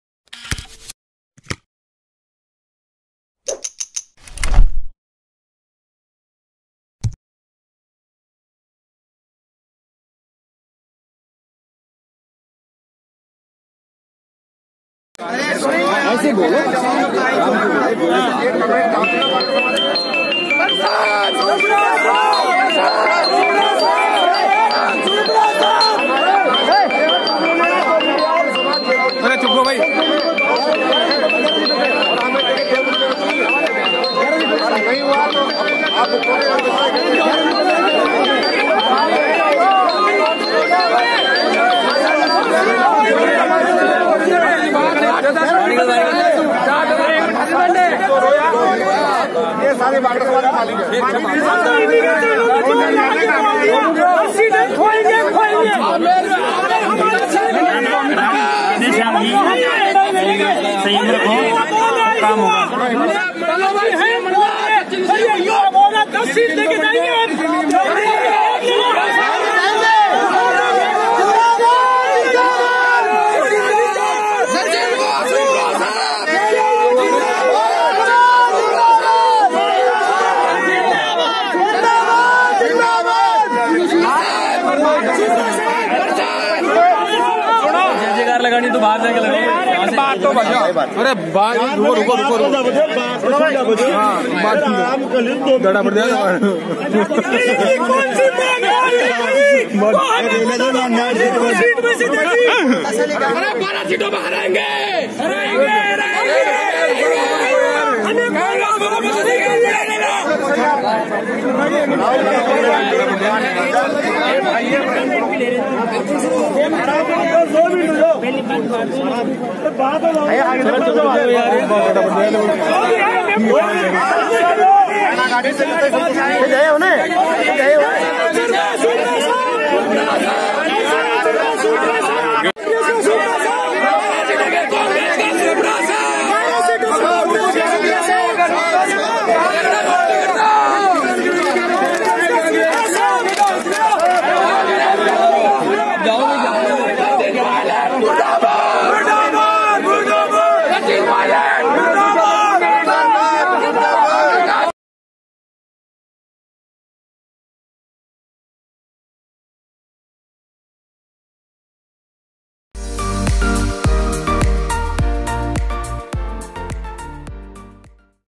PCC के बाहर PCC चीफ सचिन पायलट ओर प्रशांत शर्मा के खिलाफ प्रदर्शन करते बागड़ा ब्राह्मण के लोग....
People from Bagra Brahmin performing against PCC Chief Sachin Pilot and Prashant Sharma outside PCC .